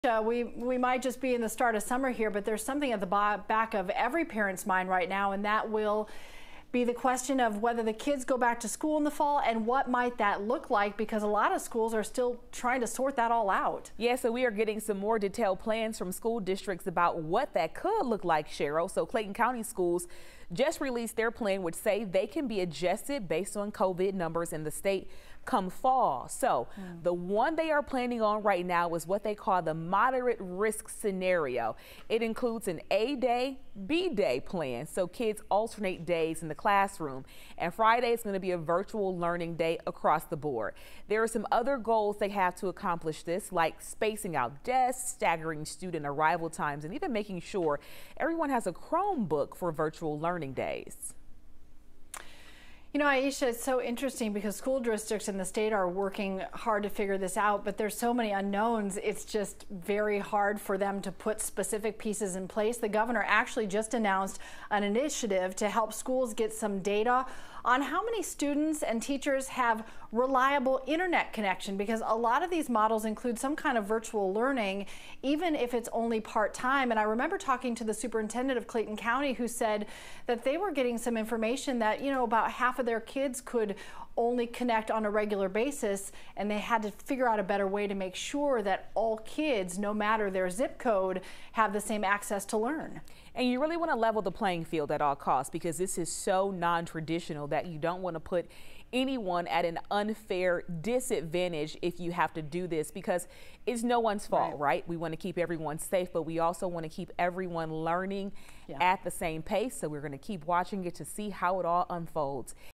Clayton County Public Schools releases plan for returning to classrooms
They said they plan can be adjusted based on COVID-19 numbers.